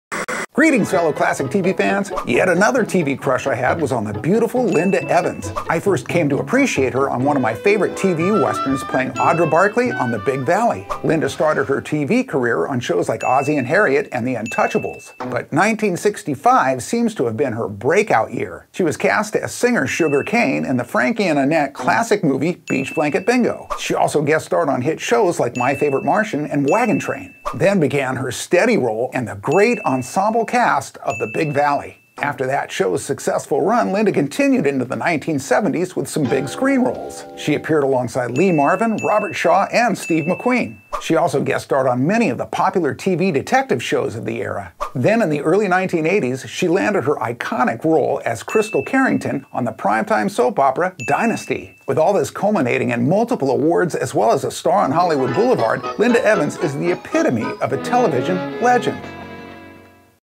LINDA EVANS! Stunning beauty and talent combined. Retro TV Trivia takes a closer look!
In her iconic role in classic television's TheBigValley as Audra Barkley, LindaEvans caused me (and countless others) to develop a major TV crush on her! And of course, she continued on with yet another iconic role as Krystal Carrington opposite co-stars John Forsyth and Joan Collins on Dynasty.
It's well over 40 interviews with some of your favorite classic TV icons!